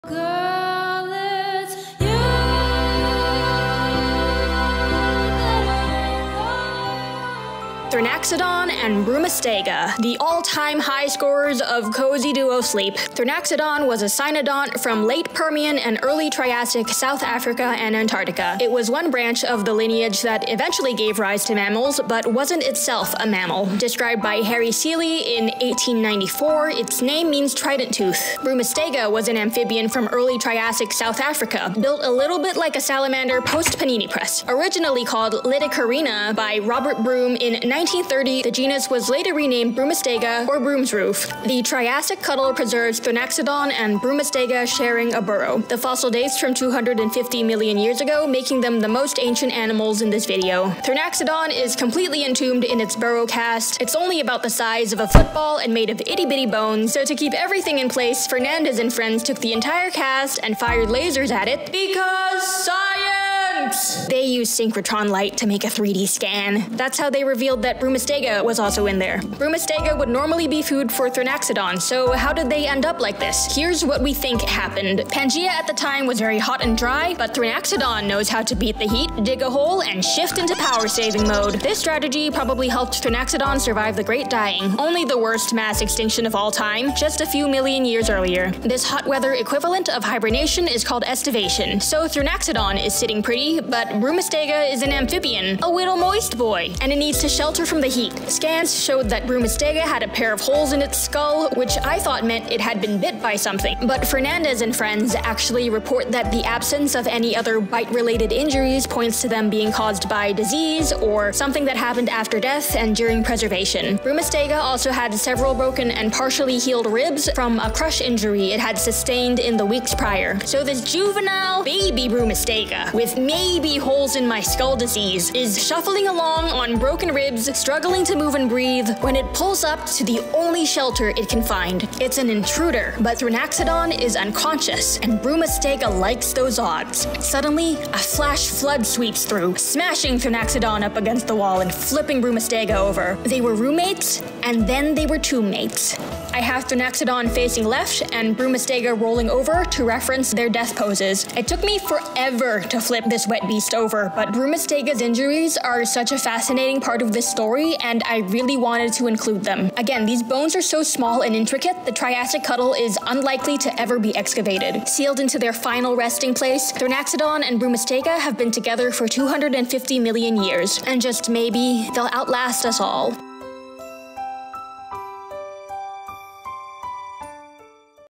TRIASSIC CUDDLE HOURS
Check out Fernandez et al’s 2013 paper, “Synchrotron Reveals Early Triassic Odd Couple: Injured Amphibian and Aestivating Therapsid Share Burrow” for more on how paleontologists ruled out the other possibilities for how Broomistega and Thrinaxodon ended up sharing a burrow. PBS Eons also has a great video on this topic!

MANY PAPERS ARE FREE YOU CAN JUST READ THEM AND LEARN ABOUT COOL STUFF PEOPLE ARE DOING ALL AROUND THE WORLD!! :DD

This is part 2 of 3 of the explainers accompanying my “Paleo Sleep” short.